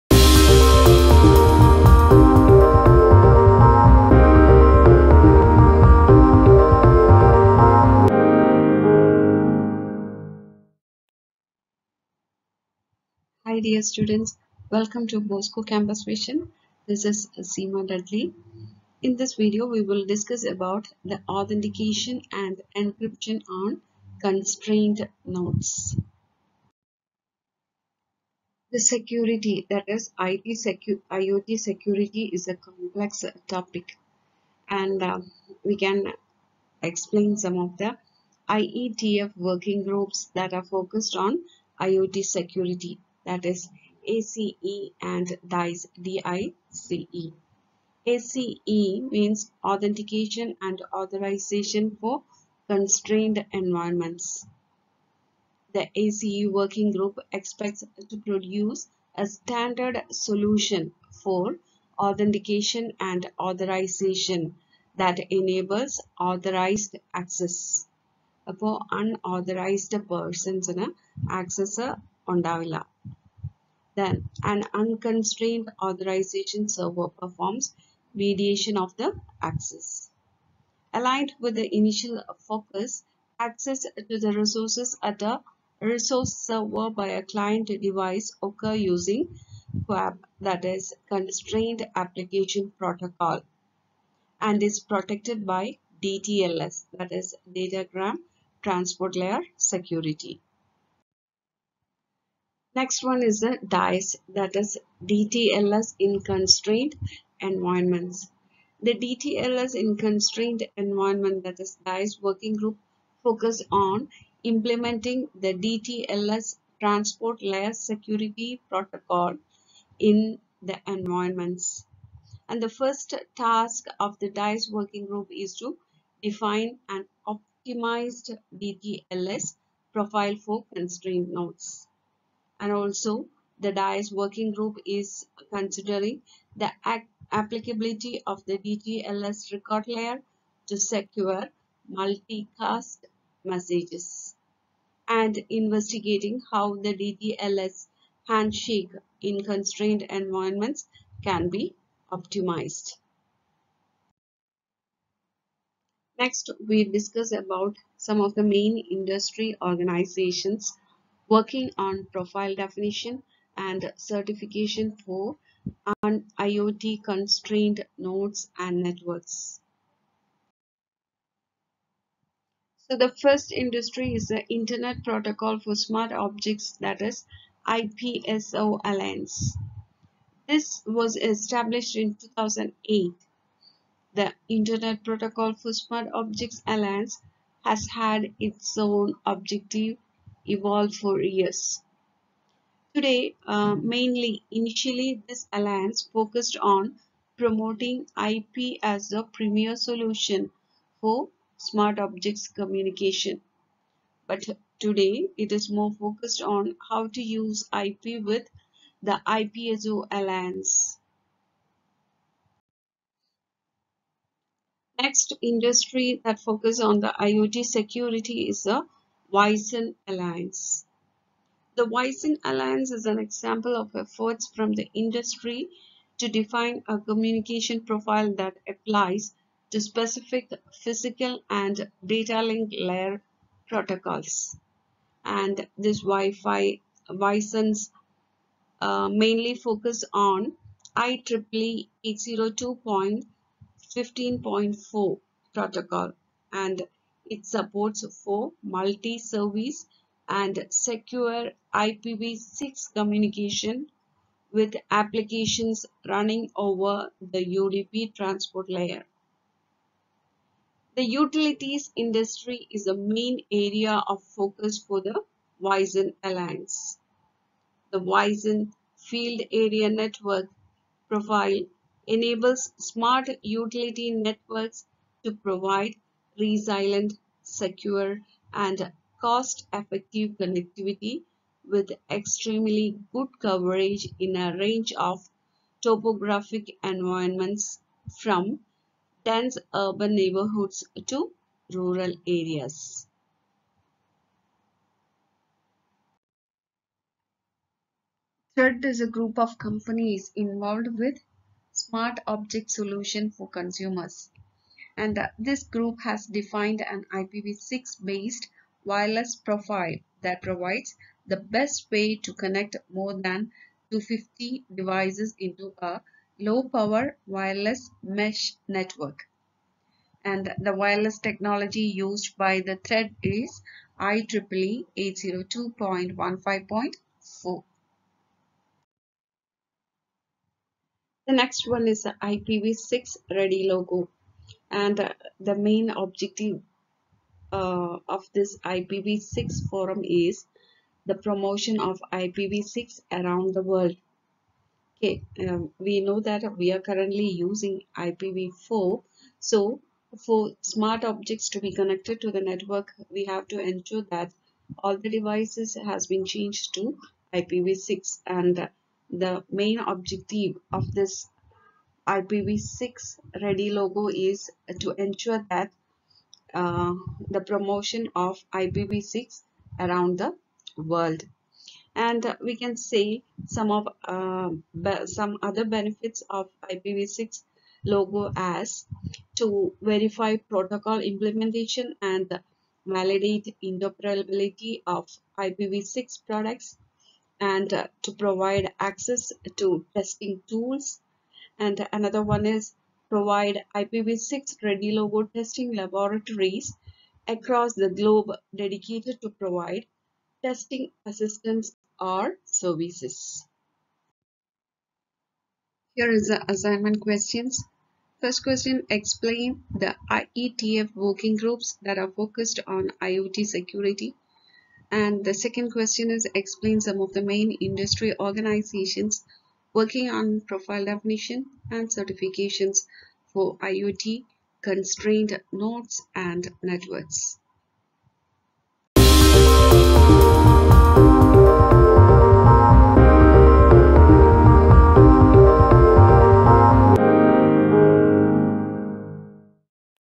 INTERNET OF THINGS LECTURE -18 - AUTHENTICATION AND ENCRYPTION ON CONSTRAINED NODES -BCA (S6)
Authentication and Encryption on Constrained Nodes
The IETF working groups that are focused on IoT security : ACE and DICE.
ACE (Authentication and Authorization for Constrained Environments )
The ACE working group expects to produce a standardized solution for authentication and authorization that enables authorized access (Get, Put, Post, Delete) to resources identified by a URI and hosted on a resource server in constrained environments. An unconstrained authorization server performs mediation of the access. Aligned with the initial focus, access to resources at a resource server by a client device occurs using CoAP (Constrained Application Protocol ) and is protected by DTLS(Datagram Transport Layer Security).